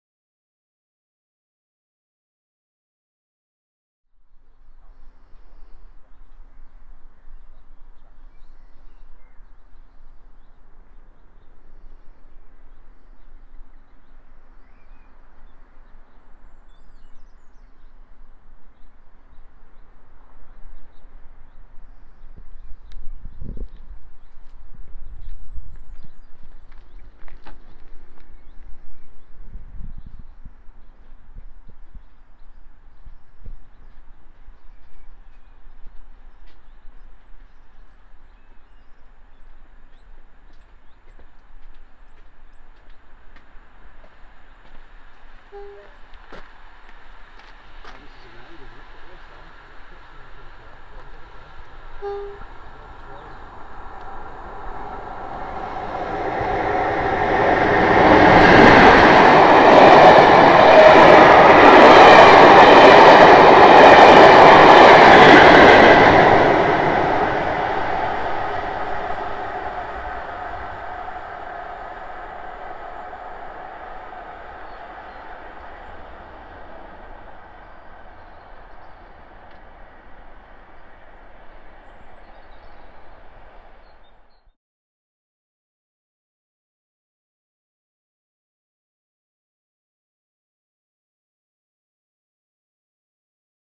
Heritage Diesel Train Settle to Carlisle and Costal Statesman D1935(47805) + 47953 Langho 06/05/2023
Heritage Diesel Train on the Settle to Carlisle and Costal Statesman with D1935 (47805) 'Roger Hosking MA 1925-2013' + 47953 'Galloway Princess' are seen passing through Langho near Blackburn on the Hellifield Line on the 6th of May 2023.